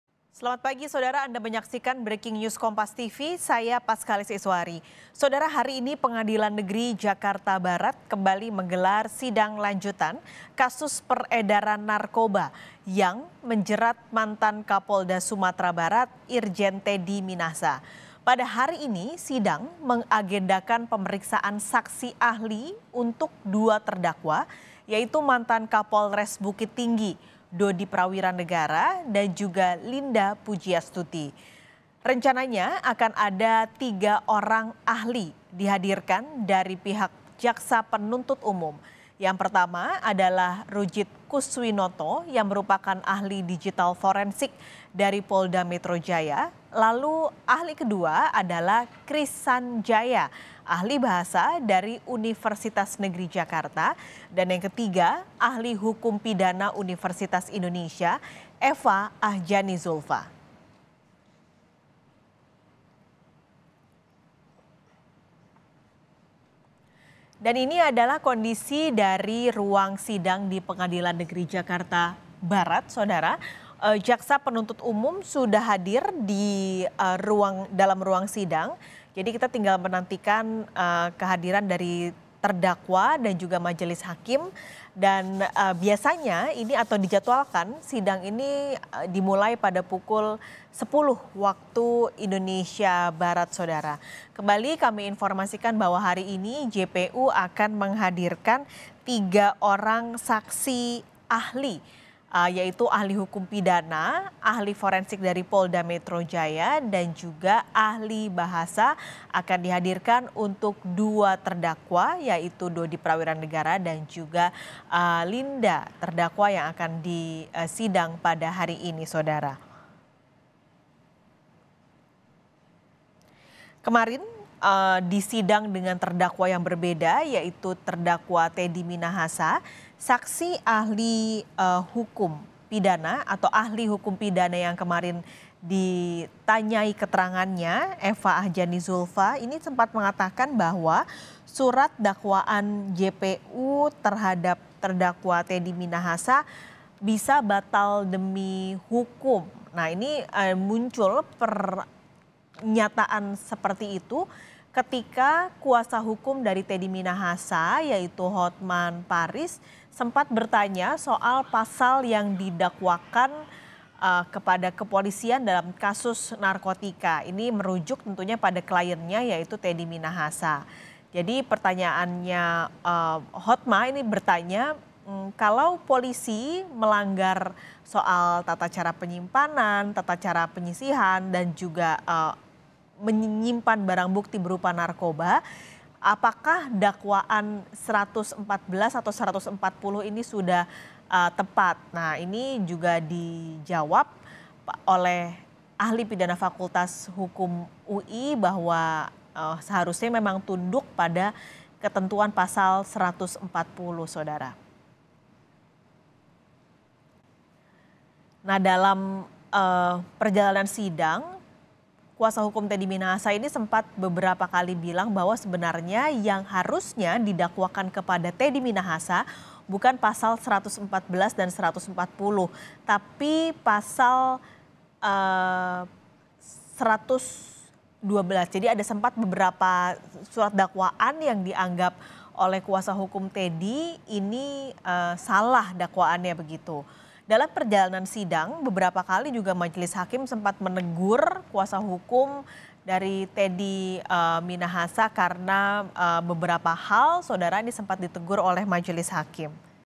Dody dan Linda Jalani Sidang Pemeriksaan Saksi Ahli
JAKARTA, KOMPAS.TV - Hari ini (8/03), Pengadilan Negeri Jakarta Barat kembali menggelar sidang lanjutan kasus narkoba yang menjerat mantan Kapolda Sumatera Barat, Irjen Teddy Minahasa.

Pada hari ini sidang menggagendakan pemeriksaan saksi ahli untuk 2 terdakwa yaitu mantan Kapolres Bukittinggi, Dody Prawiranegara dan Linda Puji Astuti.

Rencanya akan ada 3 orang ahli yang dihadirkan dari pihak JPU, di antaranya Ahli Digital Forensik Polda Metro Jaya, Ahli Bahasa Universitas Negeri Jakarta dan Ahli Hukum Pidana Universitas Indonesia.